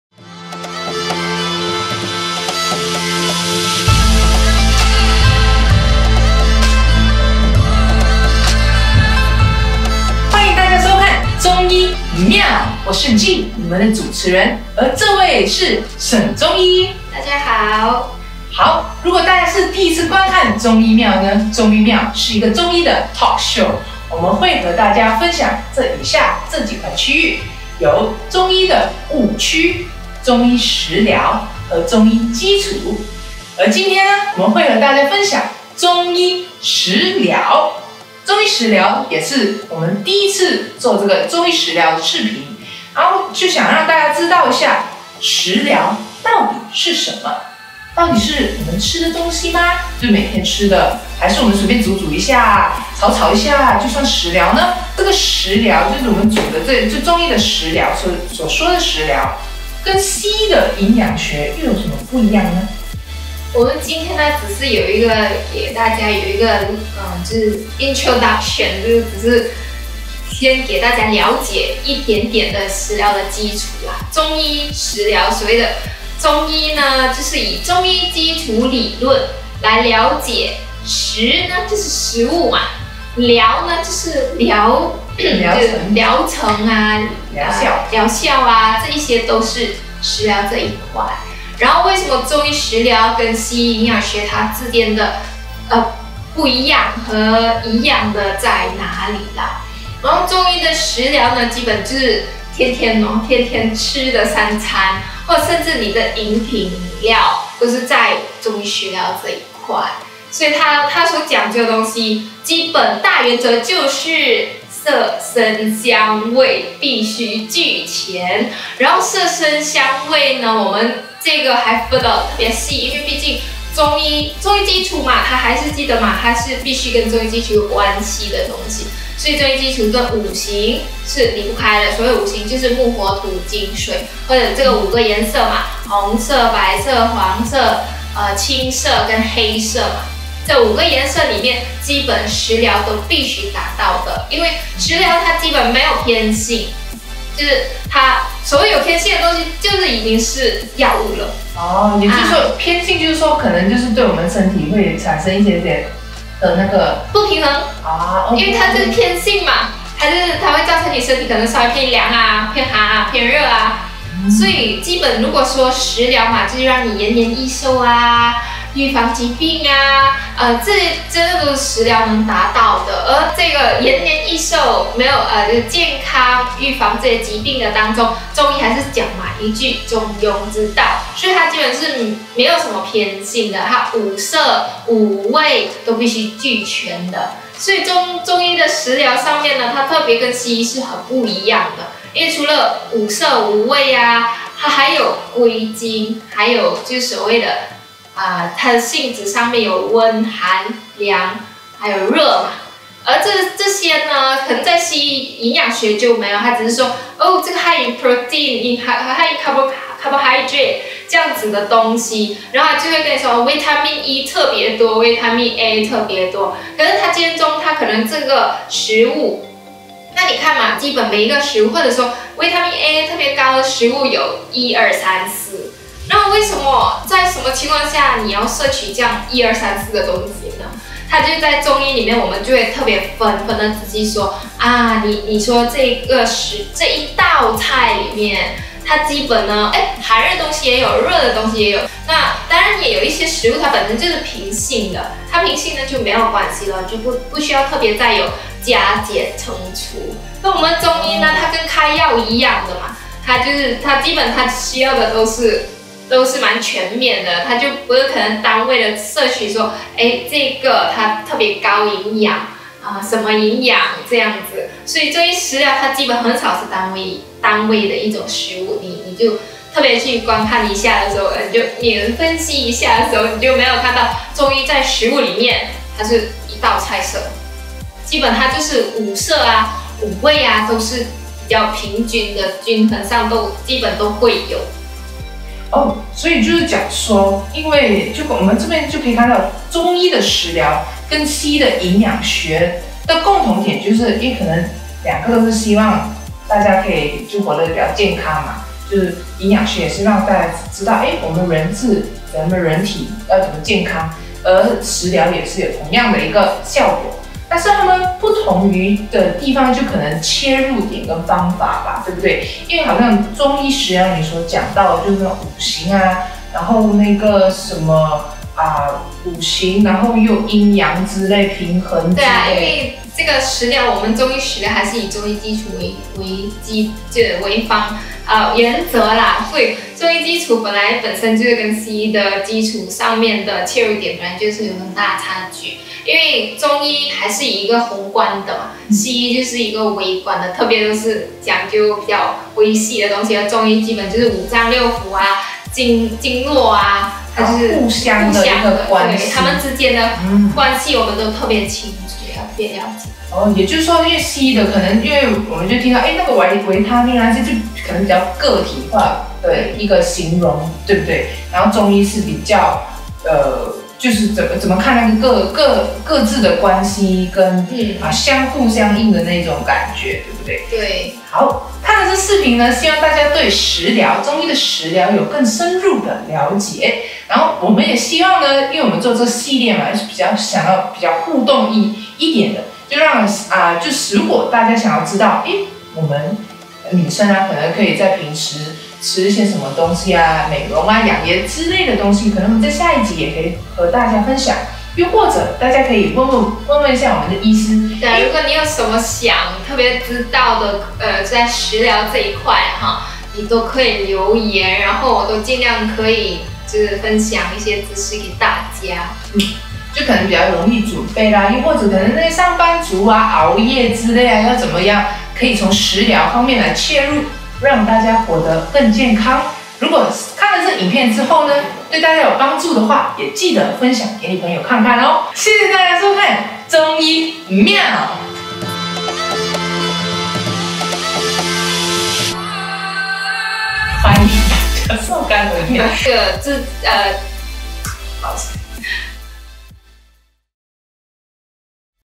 「中醫妙」中醫食療#1: 中醫食療和營養學有什麼不同？
「中醫妙」是一個讓大家能夠輕鬆、簡單了解中醫的一個talk show。也藉此機會推廣中醫，修正對中醫的誤解，讓中醫生活化。同時，呼籲大家重視養生、治療和康復，並擁有基本的思路來做有效的選擇。